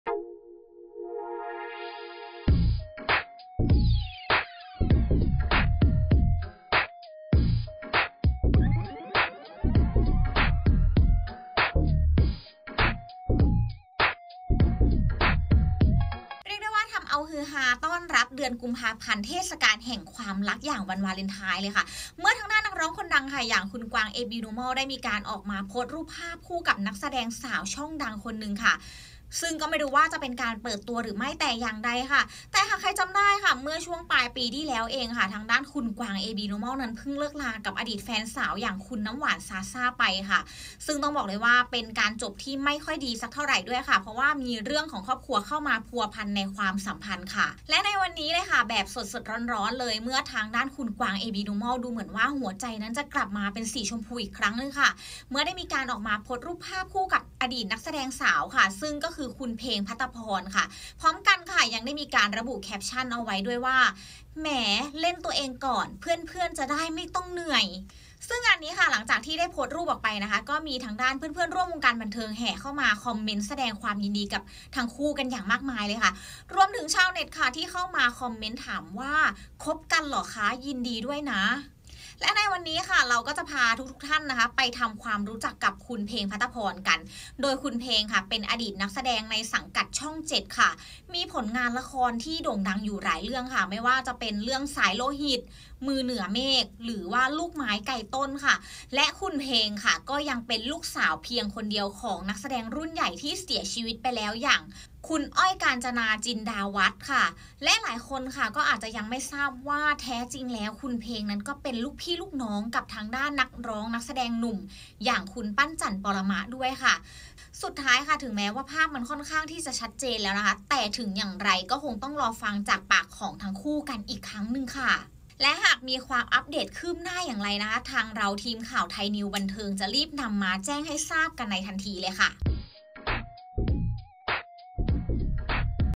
เปิดวาร์ป "เพลง ภตภร" แฟนใหม่ "กวาง ABnormal" เป็นถึงลูกสาวนักแสดงรุ่นใหญ่! | รู้กันบันเทิง
รู้กันบันเทิง : เรียกได้ว่าทำเอาฮือฮาเมื่อทางด้านนักร้องคนดังอย่าง กวาง ABnormal ได้ออกมาโพสต์รูปภาพกับนักแสดงสาวช่องดังอยาง เพลง ภตภร ซึ่งก็ไม่รู้ว่าจะเป็นการเปิดตัวหรือไม่แต่อย่างใด และหลายคนอาจจะไม่ทราบว่าแท้จริงแล้วทางด้าน เพลง ภตภร เป็นดีกรีนักแสดงรุ่นใหญ่ด้วย